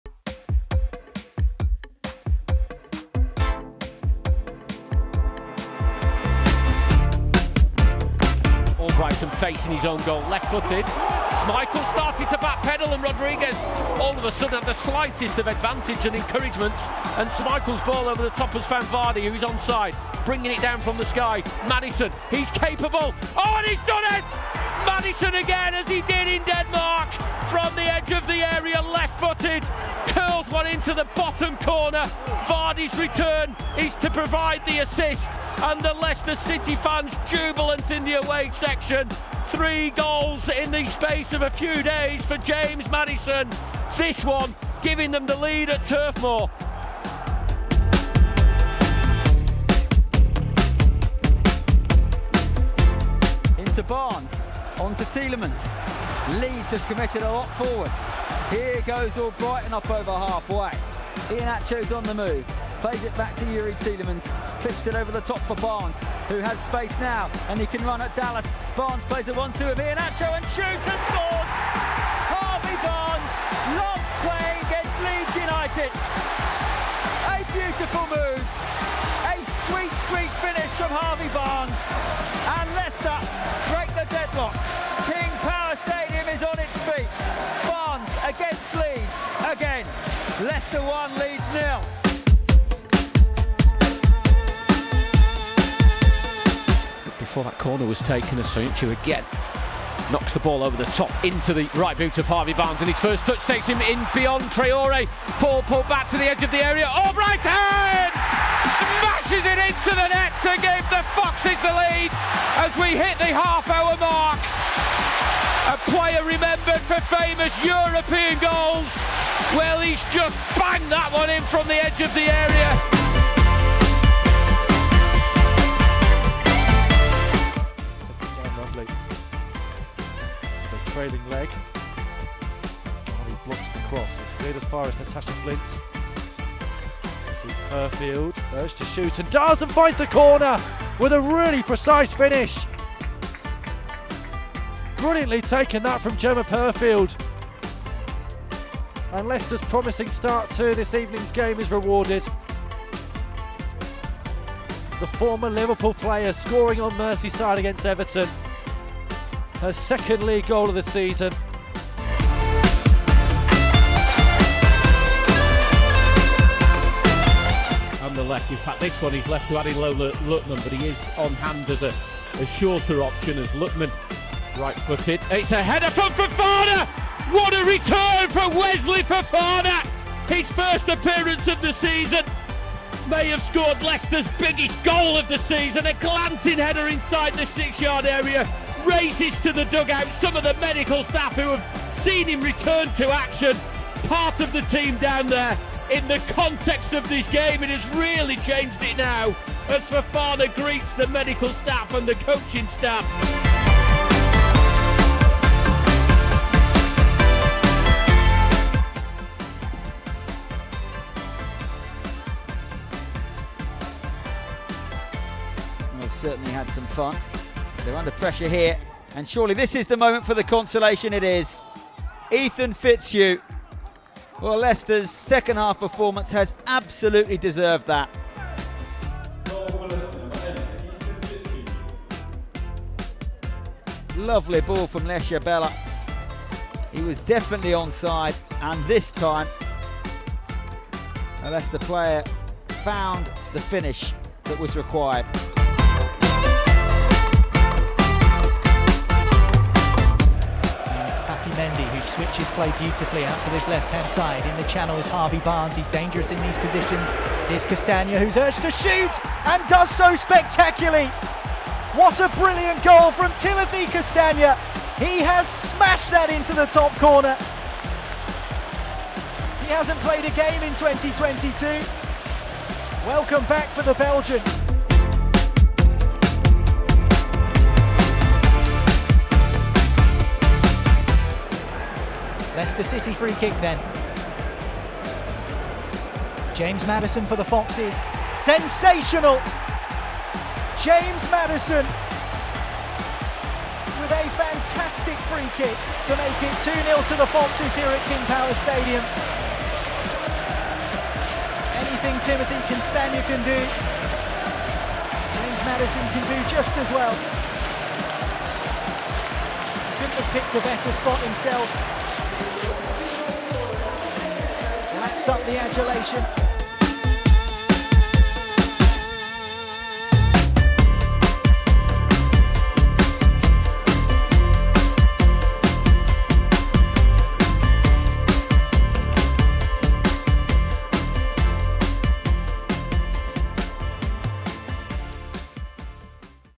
Goal Of The Month | March 2022 | Leicester City
James Maddison, Harvey Barnes, Marc Albrighton, Jemma Purfield, Wesley Fofana, Ethan Fitzhugh, Timothy Castagne all feature in the Foxes' March Goal of the Month.